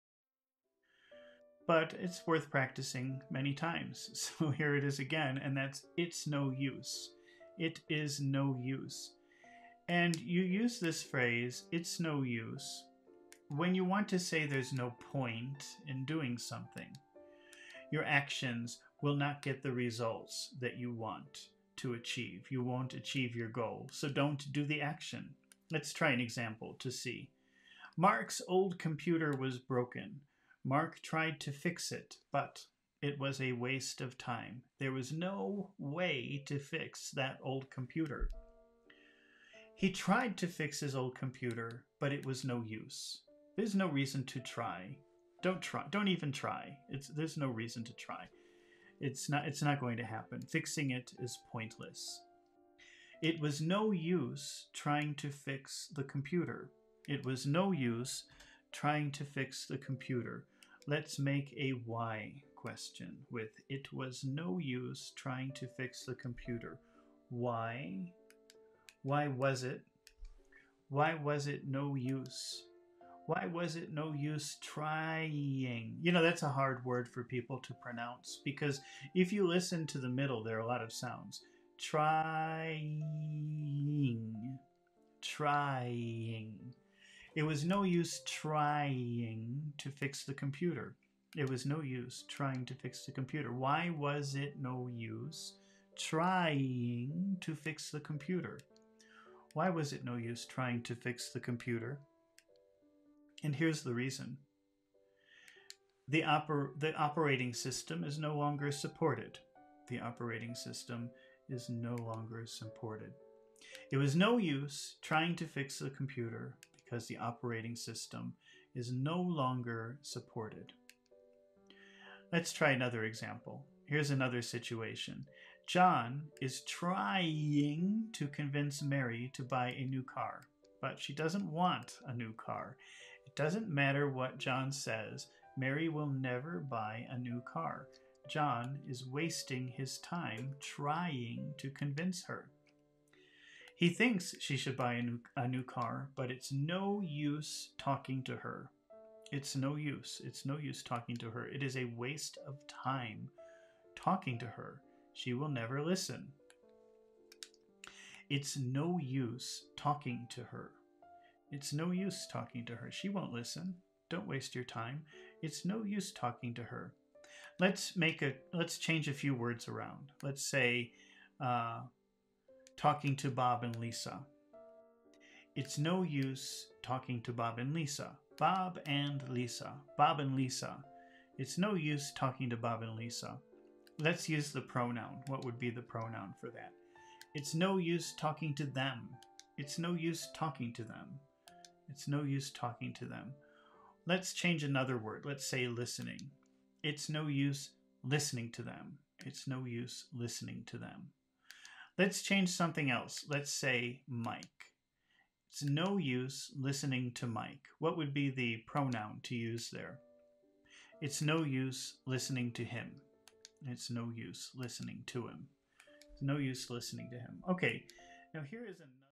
English class Idioms practice listening | speaking Fluency "IT'S NO USE..." learn it here
In this video, we practice the meaning of the expression in English 'it's no use". Then we use it in a sentence and practice the meaning with a unique listening comprehension and speaking fluency exercise.